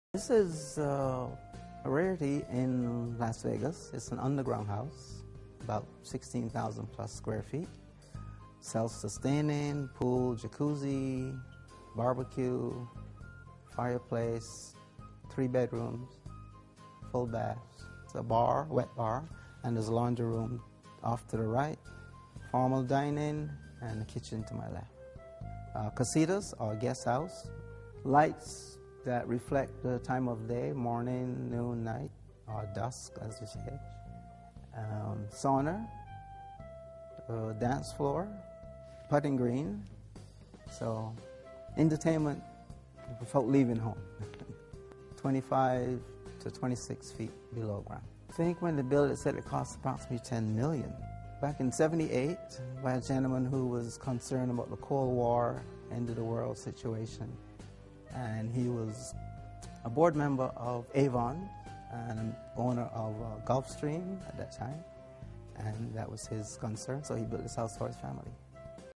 Incredible underground 'mansion style' Las Vegas fallout bunker
A fallout bunker made to look like an outdoor mansion underground in Las Vegas has gone on the market. A snip at only $1.7 million.